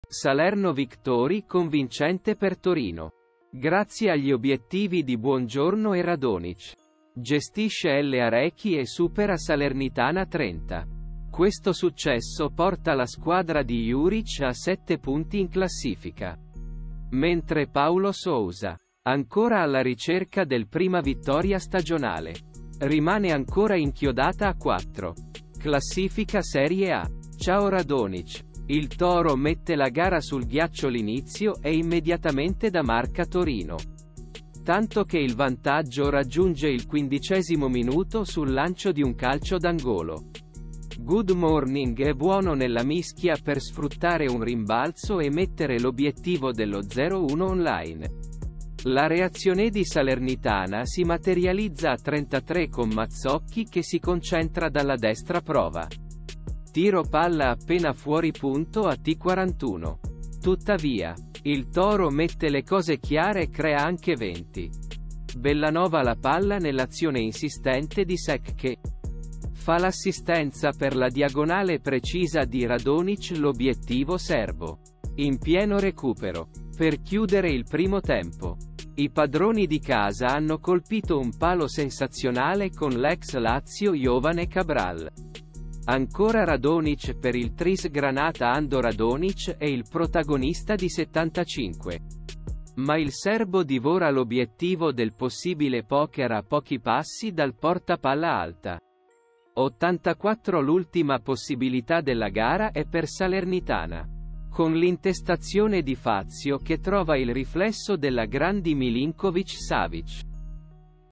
Radonjic show, il Torino travolge la Salernitana
Salerno - Victory convincente per Torino, grazie agli obiettivi di Buongiorno e Radonjic , gestisce l'Arechi e supera Salernitana 3-0. Questo successo porta la squadra di Juric a 7 punti in classifica, mentre Paulo Sousa, ancora alla ricerca delPrima vittoria stagionale, rimane ancora inchiodata a 4. Classifica Serie A.  Ciao-radonjic, il toro mette la gara sul ghiaccio L'inizio è immediatamente da Marca Torino, tanto che il vantaggio raggiunge il 15 ° minuto sul lancio di un calcio d'angolo, Good Morning è buono nella mischia per sfruttare un rimbalzo e mettere l'obiettivo dello 0-1 online. La reazionedi Salernitana si materializza a 33 'con Mazzocchi che si concentra dalla destra prova tiro palla appena fuori.at 41', tuttavia, il toro mette le cose chiare crea anche 2-0.Bellanova la palla nell'azione insistente di Seck che faL'assistenza per la diagonale precisa di Radonjic l'obiettivo serbo. In pieno recupero, per chiudere il primo tempo, i padroni di casa hanno colpito un palo sensazionale con l'ex Lazio Jovane Cabral. Ancora radonjic per il tris granata Ando Radonjic è il protagonista di 75, ma il serbo divora l'obiettivo del possibile poker a pochi passi dalPorta palla alta. 84 'L'ultima possibilità della gara è per Salernitana, con l'intestazione di Fazio che trova il riflesso della Gran di Milinkovic-Savic.